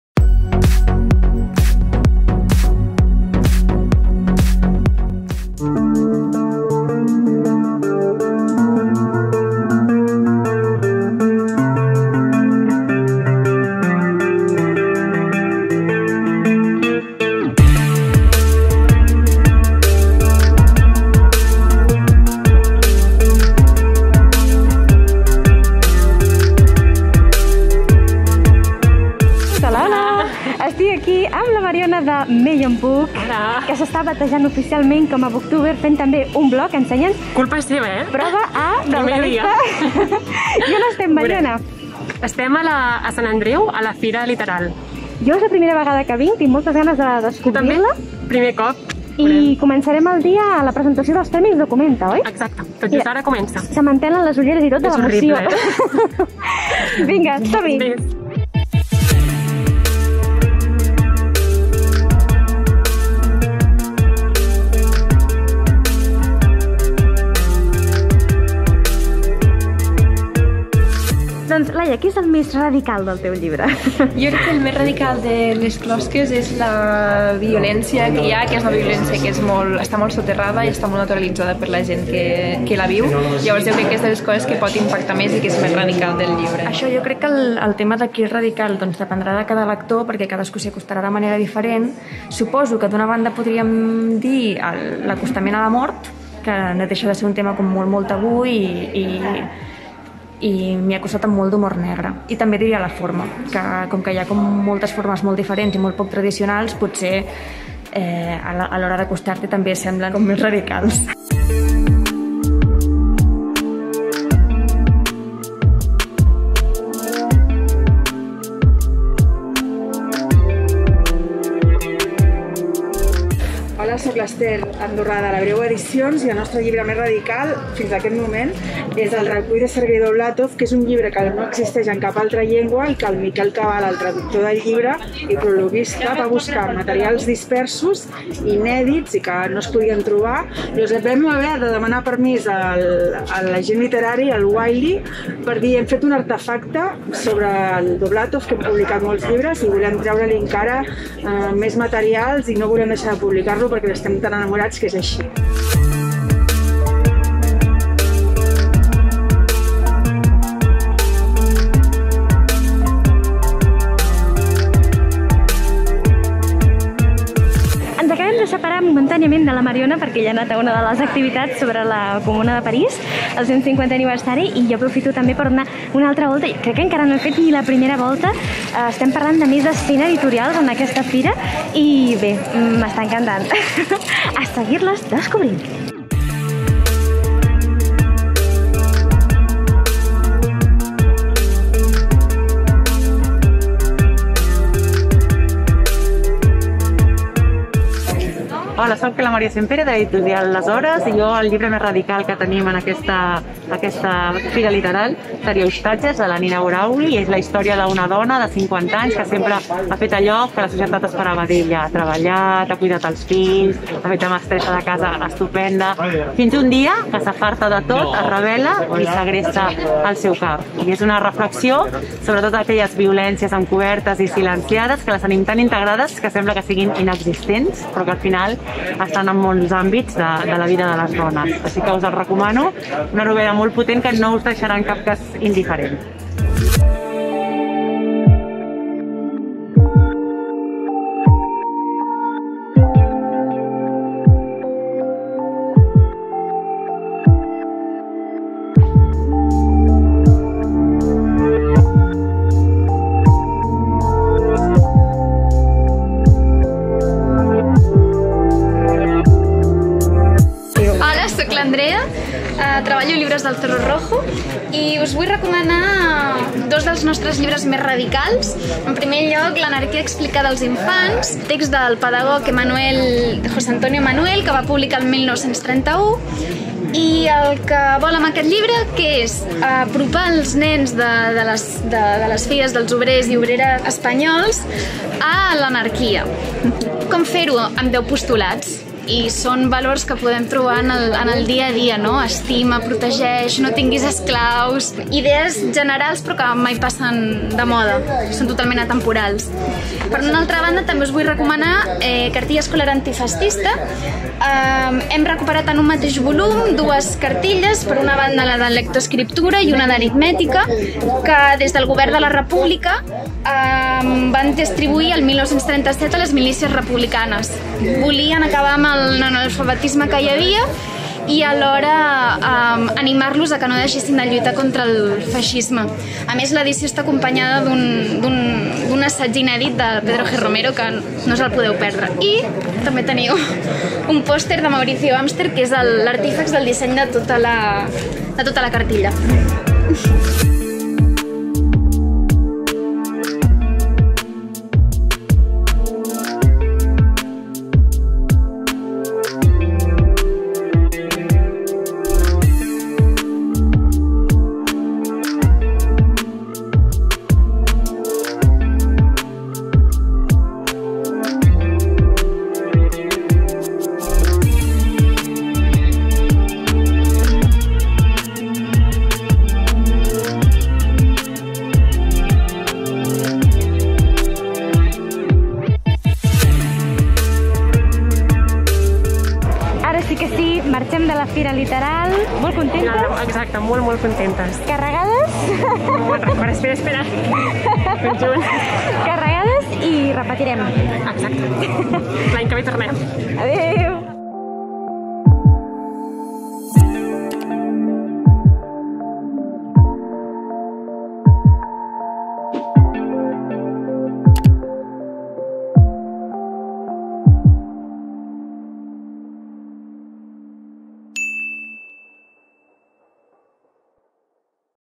Fira Literal 2021 (VLOG)
Els dies 22 i 23 de maig va tenir lloc la 7a edició de la Fira Literal a la Fabra i Coats de Barcelona. Unes 7.000 persones la van visitar al llarg del cap de setmana, incloses la Mariona (del canal Meyonbook) i jo!

Mx